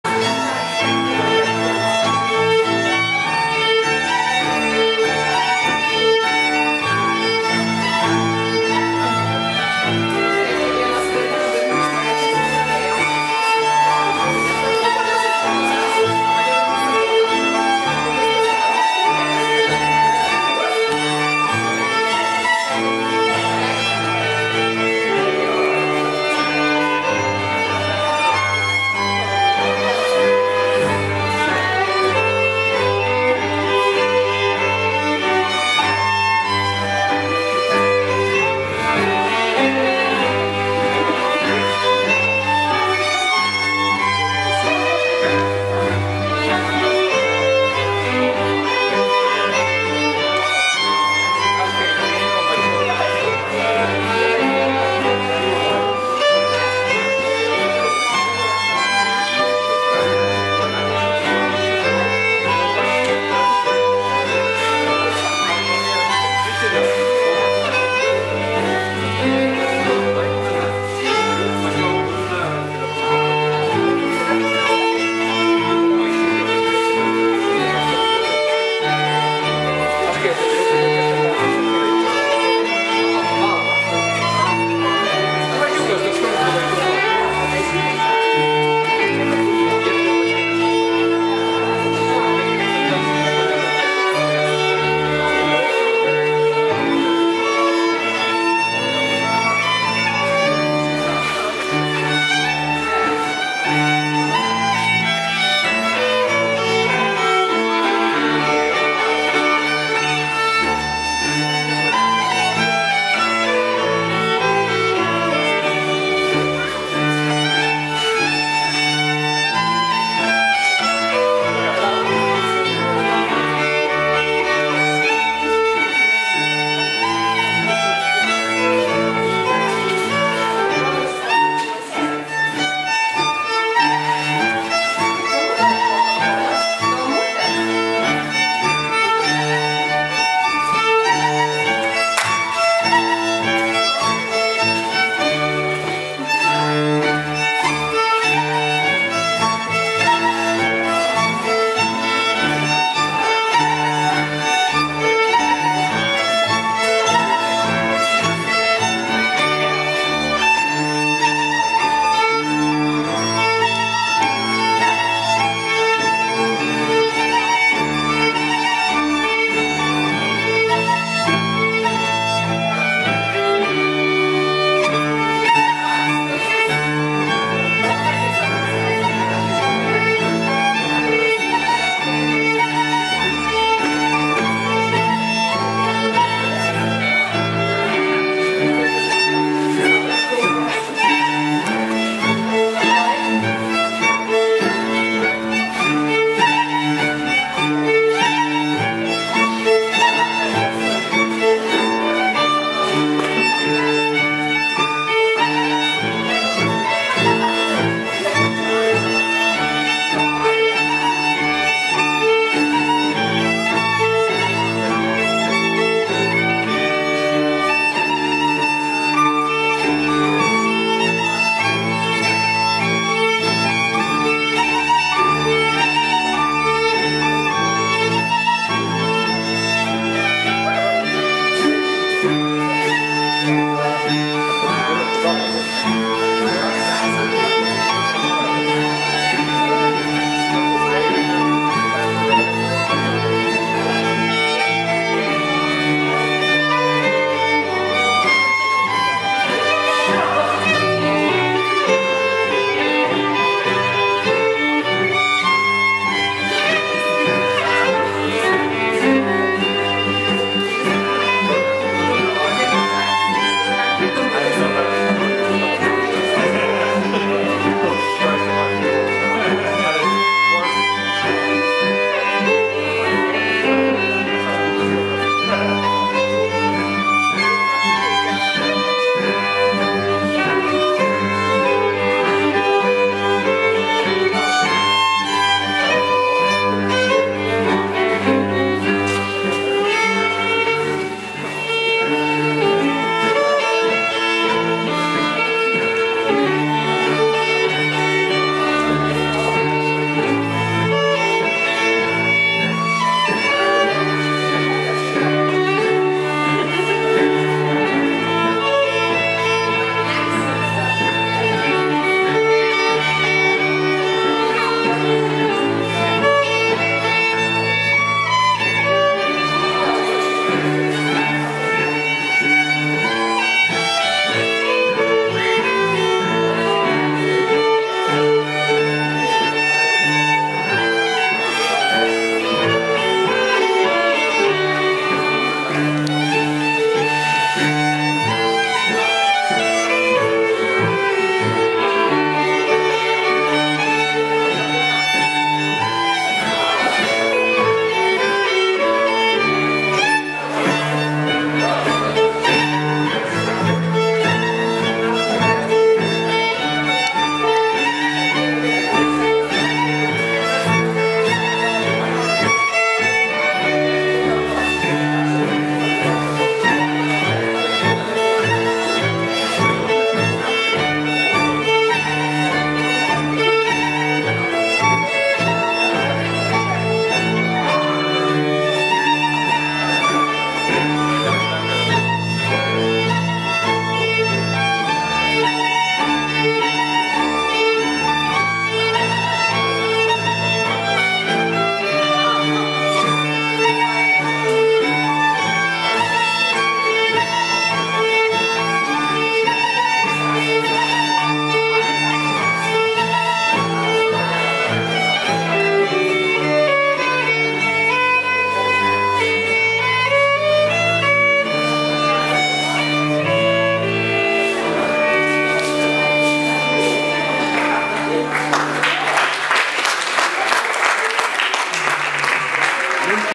Hermann Fritz & co
Hermann Fritz, Anna, Wanda Leben & Anton Hacker in Lithuanian dance camp in Dreverna 2017